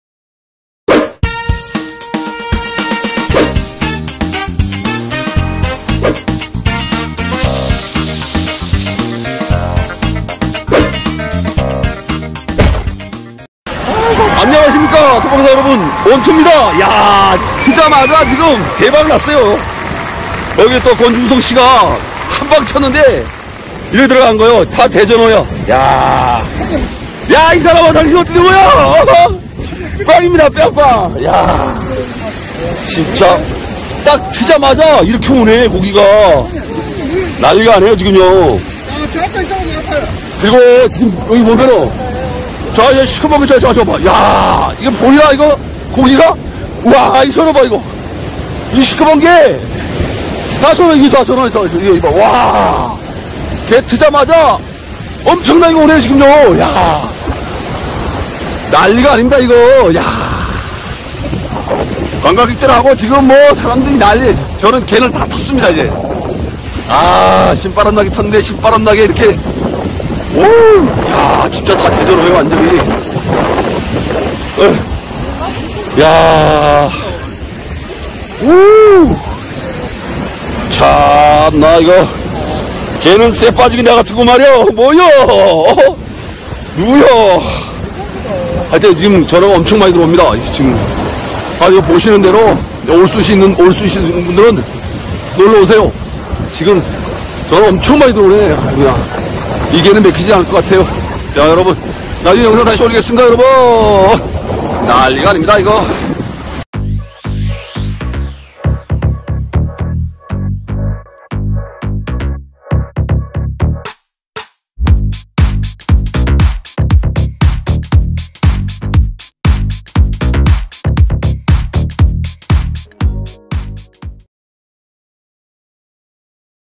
화진포해변 갯터짐 후 한번 던진 투망에 대전어 50마리에서 100마리 조과 cast net fishing เหวี่ยงแหจับปลา 봄전어투망파티 그물낚시 바다투망달인 원투형님
4월 16일 토요일 화진포해변 갯터짐 후 수천마리 추정 대전어 떼가 해안가를 따라 이동하며 기수역에 몰려듭니다...투망 한망에 수십마리에서 백마리까지 잡히는 엄청난 날이였습니다...영상속에서 대전어투망을 치신 모델은 학꽁치투망달인 권준성 투망사님...일명 권고수이십니다...^^